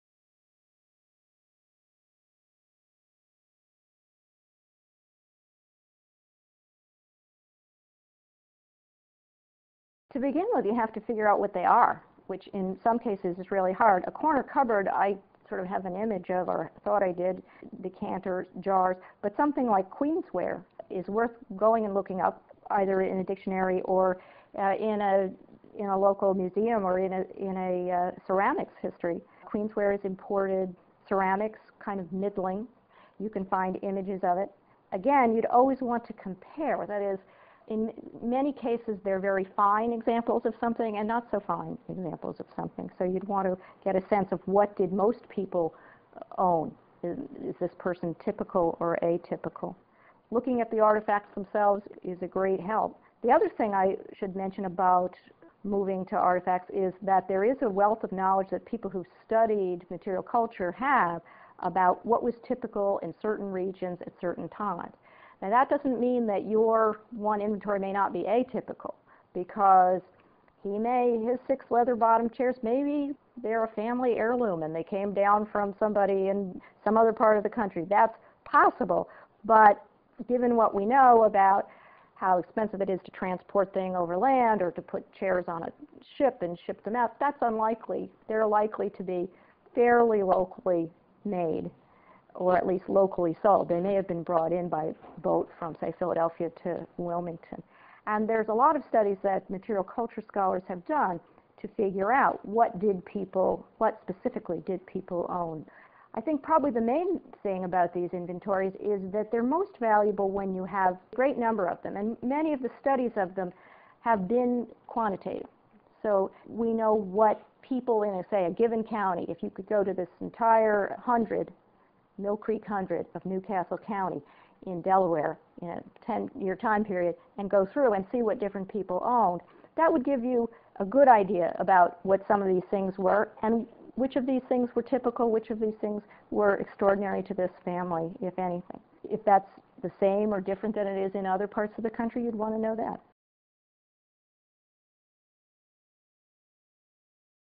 Examples of Historical Thinking - 1804 Inventory - Part Three
An inventory—a list of someone's belongings made at his or her death—can tell you something about a person's life. But what does it leave out? Barbara Clark Smith examines an 1804 inventory, asking what it does and does not record.